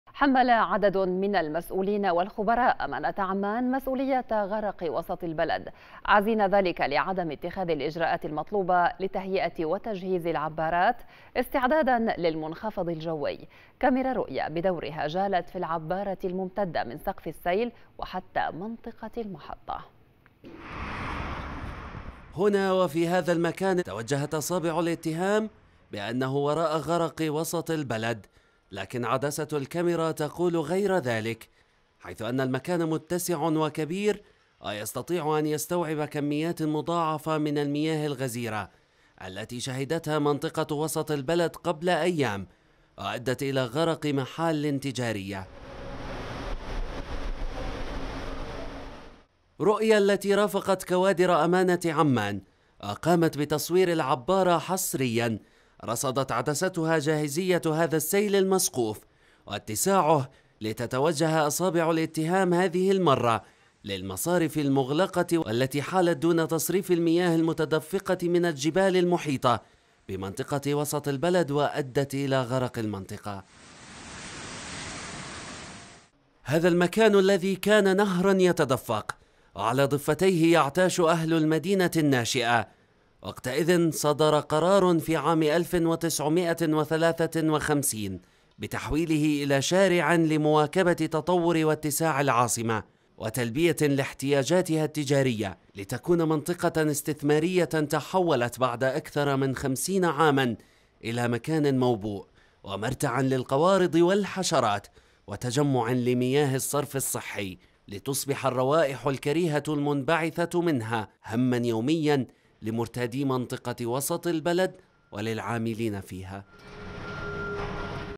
رؤيا ترصد جاهزية العبّارة "المتهمة" بغرق وسط البلد - (10-3-2019)
حمَّلَ عددٌ من المسؤولينَ و الخبراءِ أمانةَ عمانَ مسؤوليةَ غرقِ وسطِ البلد، عازينَ ذلكَ لعدمِ اتخاذِ الإجراءاتِ المطلوبة لتهيئةِ و تجهيزِ العبّارات، استعدادًا للمنخفضِ الجوي، كاميرا رؤيا بدورِها جالتْ في العبّارةِ الممتدةِ من سقفِ السيل و حتى منطقةِ المحطة.